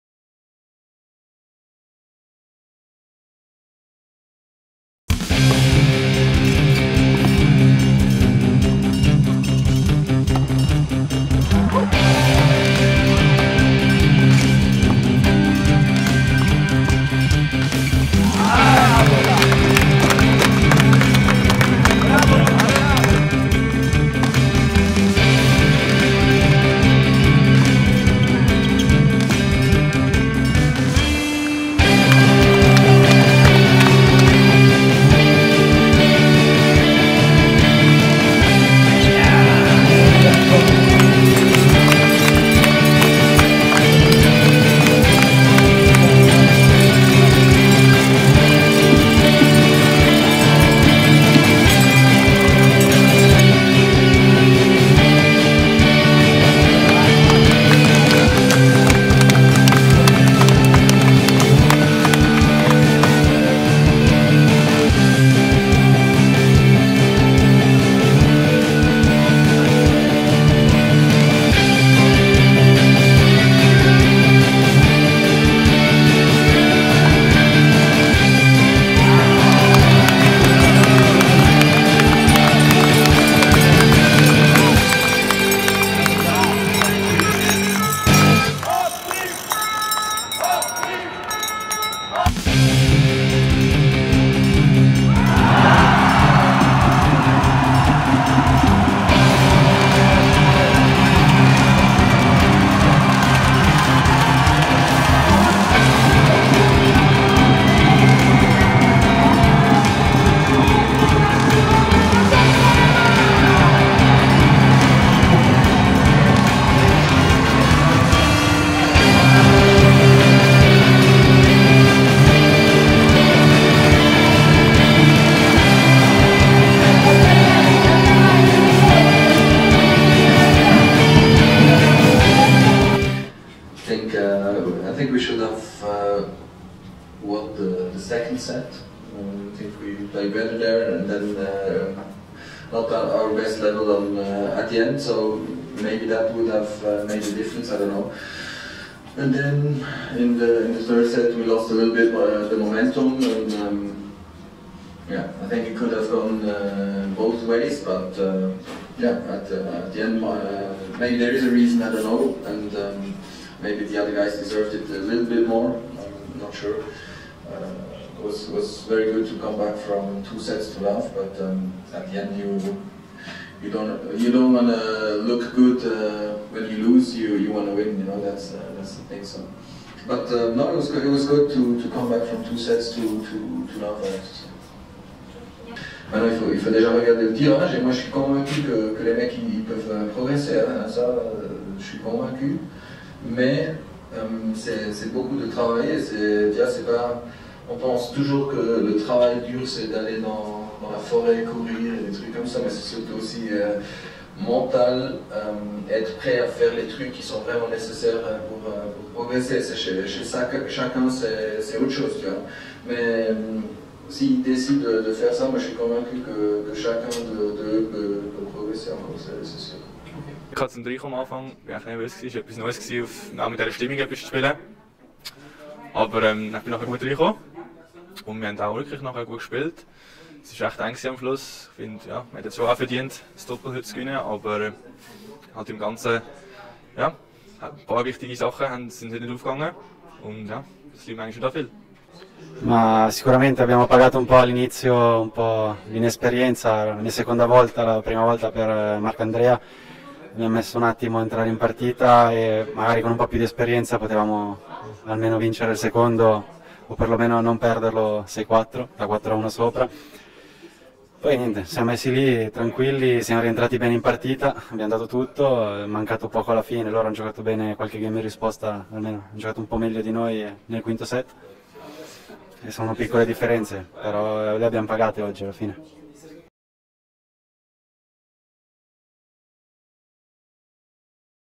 Day 2 Davis Cup tie KAZSUI
Switzerland lost the doubles match and therefore also the tie. Reactions from captain Severin Lüthi and players Luca Margaroli and Marc-Andrea Hüsler